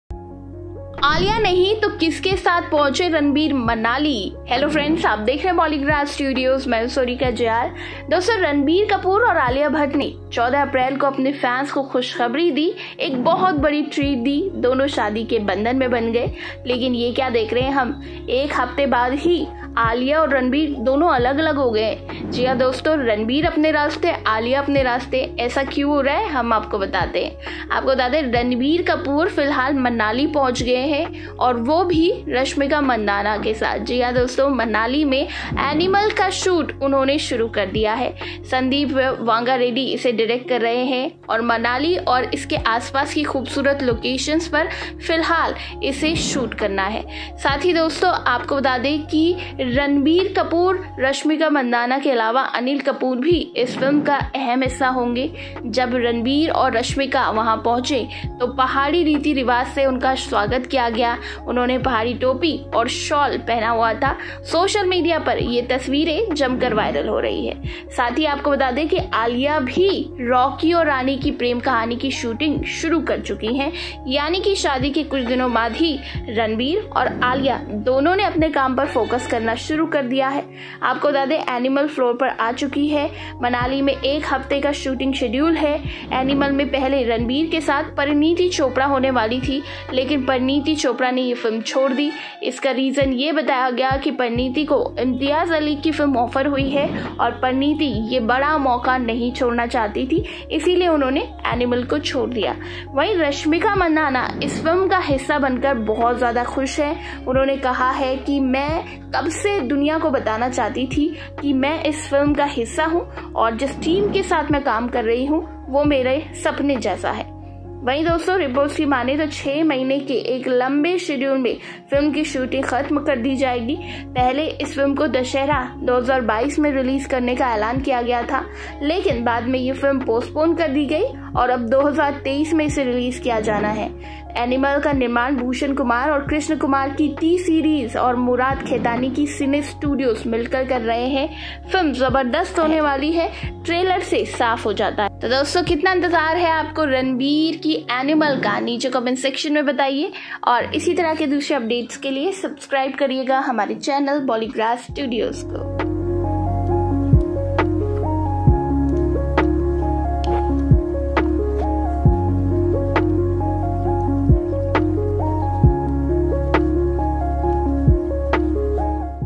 Ranbir Kapoor is booked till 2023: Here's his work list | Brahmastra | Animal | Shamshera
Recent News -Ranbir Kapoor is booked till 2023: Here's how his work slate looks like
0:00 Intro
1:30 Kabh release hogi animal movie?
2:00 Kya alia ke bina gaye manali?

Newly married Ranbir Kapoor, along with National crush Rashmika Mandanna, has reached Manali to shoot for their upcoming crime thriller Animal, to be directed by Sandeep Reddy Vanga. Pics of the two have been doing rounds on social media in which they can be seen posing with their fans. A new BTS video has surfaced on the internet where thousands of guns can be seen on the shooting spot of Animal. Ranbir Kapoor is considered to be a chocolaty romantic type hero, but going by this video, Vanga seems to have planned something very brutal that’s going to make the masses go crazy.

It would be interesting to see Ranbir Kapoor in a monster-like avatar ala Rocky Bhai from the KGF series. This would be Sandeep Vanga’s second Hindi project after the record-breaking remake of Arjun Reddy, Kabir Singh. Anil Kapoor and Bobby Deol are also a part of the project, which is produced by Bhushan Kumar, Murad Khetani’s Cine1 Studios, and Pranay Reddy Vanga’s Bhadrakali Pictures. ‘Animal’ is scheduled to release during the Independence day weekend next year.

Ranbir Kapoor and Rashmika Mandanna recently jetted off to Manali to shoot for their upcoming film, Animal. A directorial venture of Sandeep Reddy Vanga, the film also stars Bobby Deol and Anil Kapoor. Ranbir and Rashmika were spotted on the first day of shoot by a fan and now their look is going viral on social media.

Thumbnail- Nikhil
Uploaded by- Khushi Jain